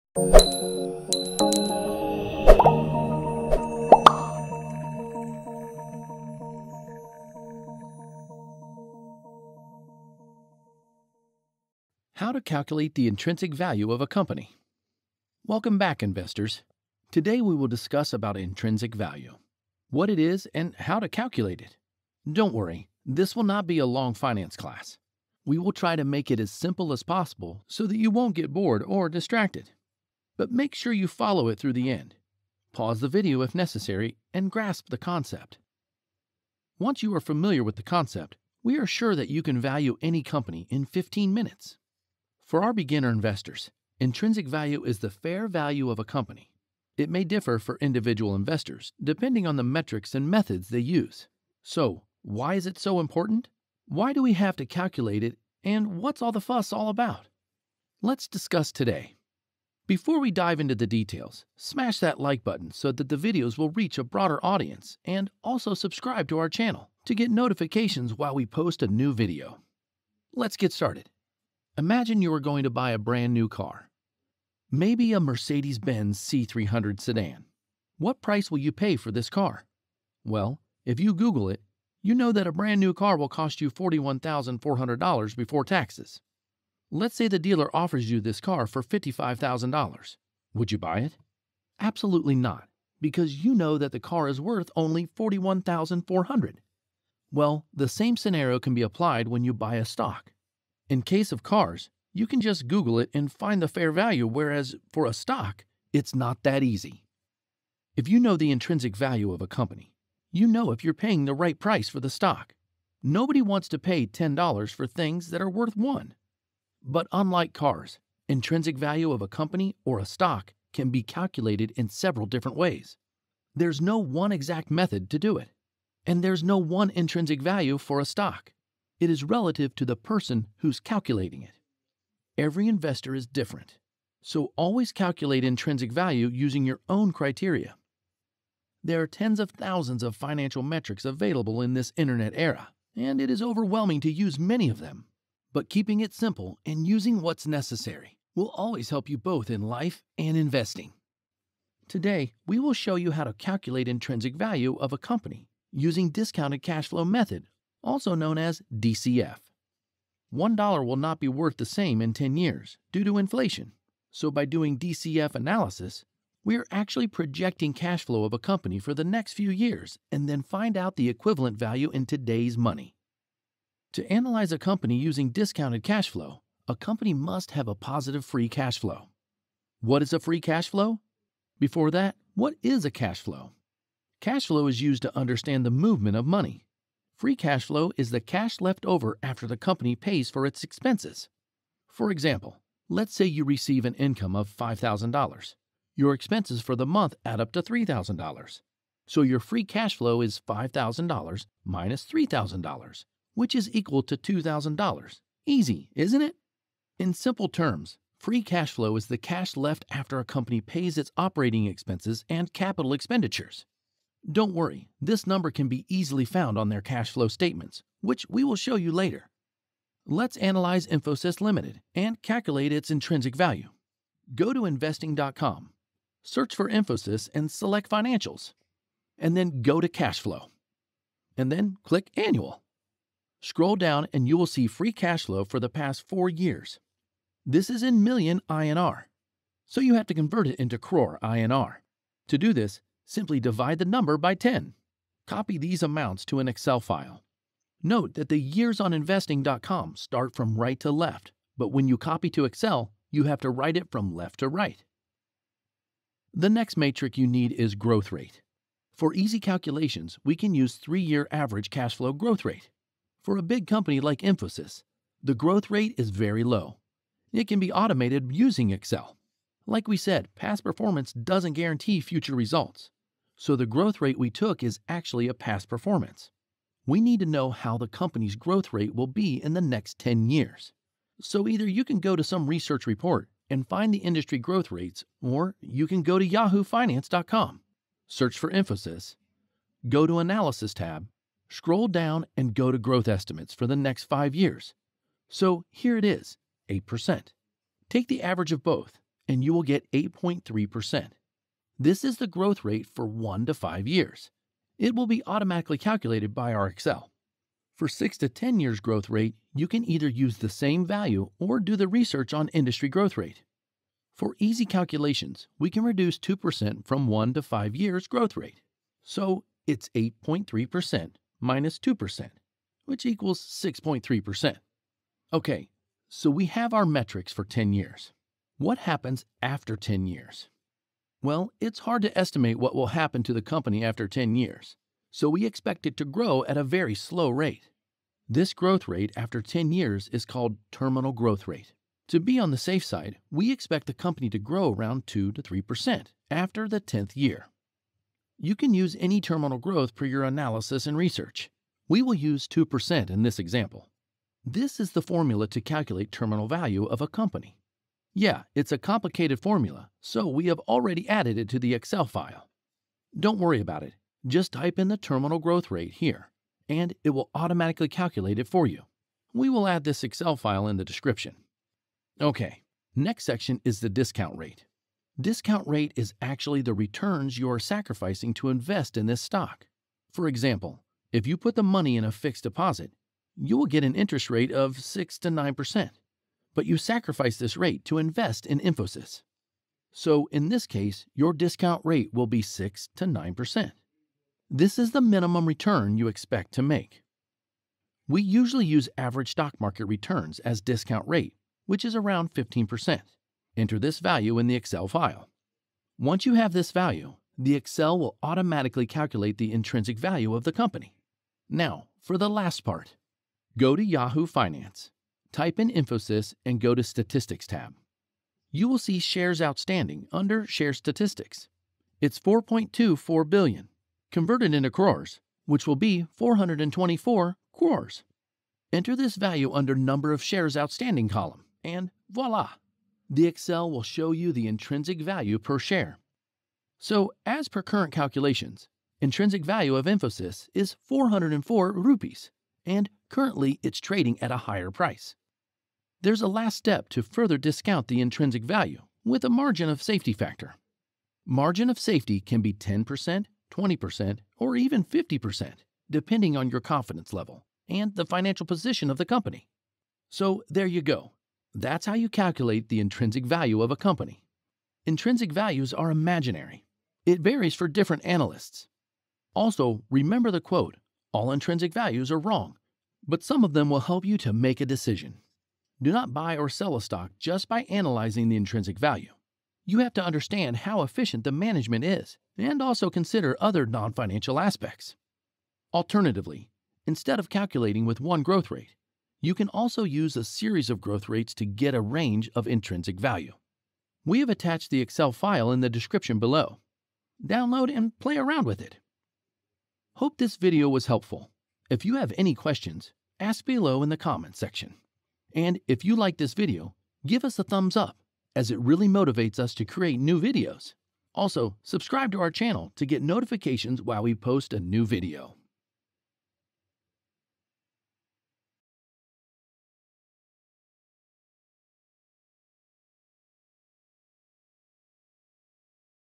Use This Method To Calculate Intrinsic Value Of a Company/Stock (With Infosys Example)
This video explains how to calculate intrinsic value of a company using a discounted cash flow or DCF method. It's one of Warren Buffet's favorite way to analyze a stock based on its future cash flow projections.